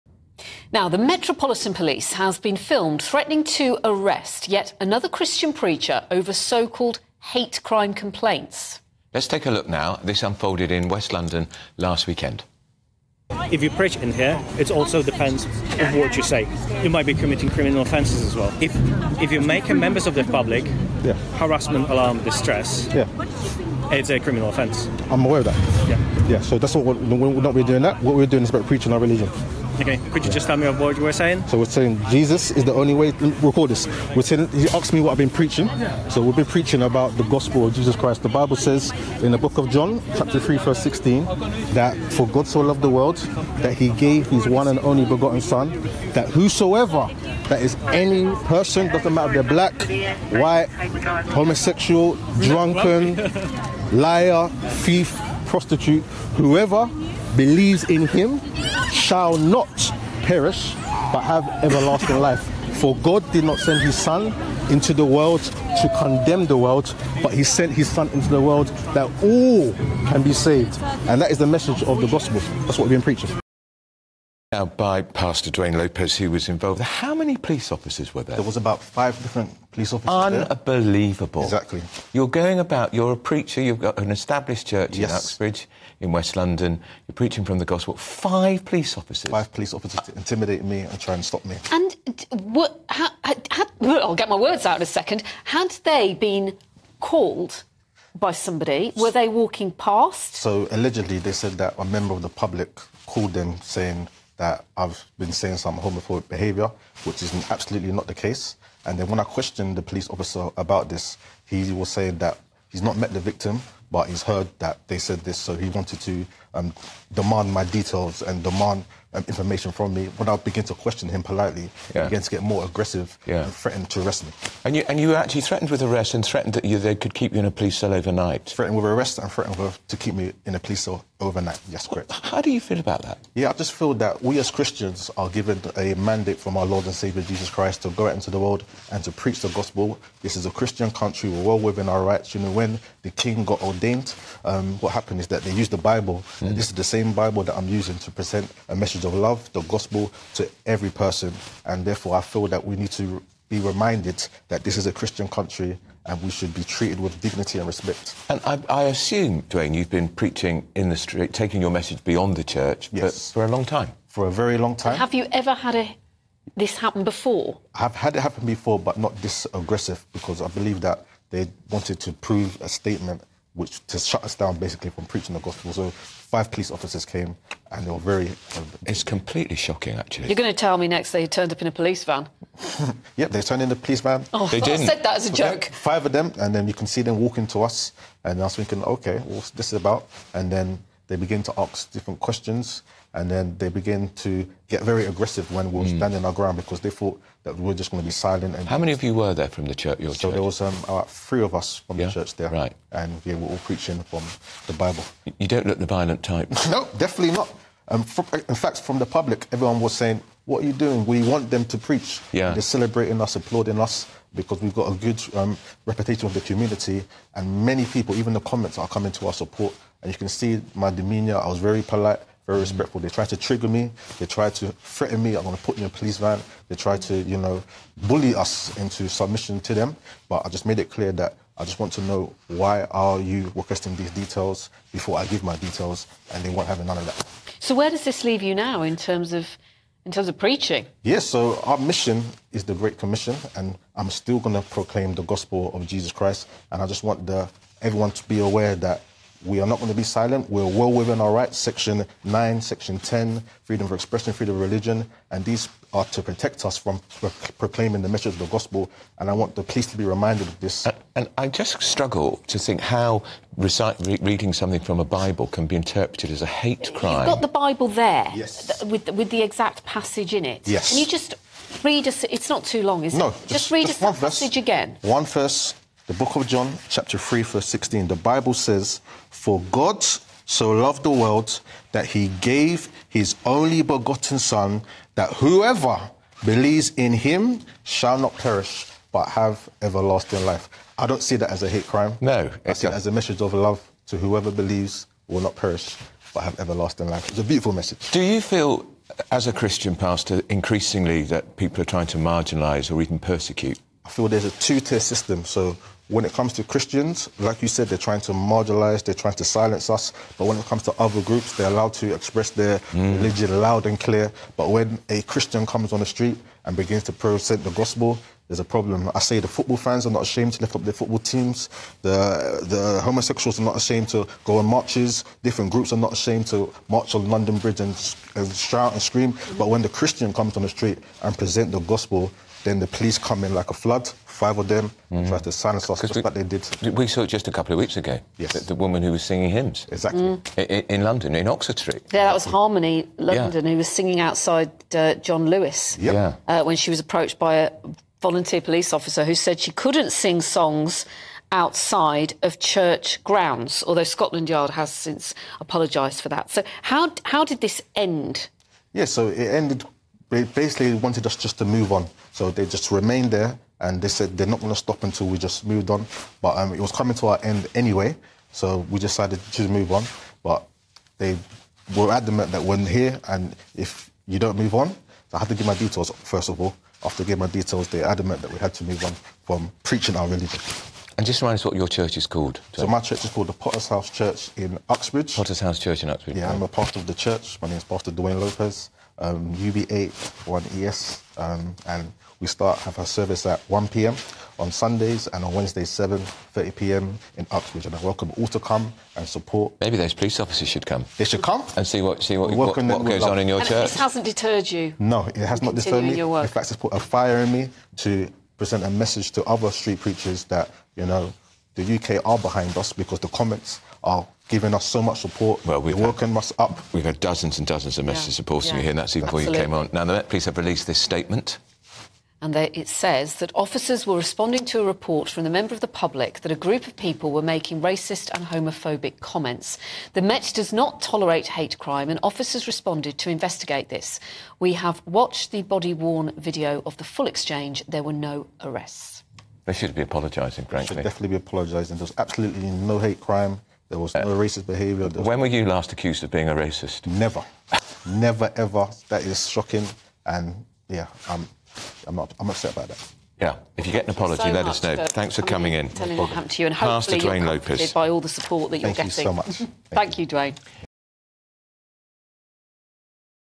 'We're a Christian country!' Gospel preacher THREATENED with arrest for hate crime by singing hymns
'This is a Christian country and we should be treated with dignity and respect.'

Gospel Preacher threatened with arrest, Dwayne Lopez, speaks out.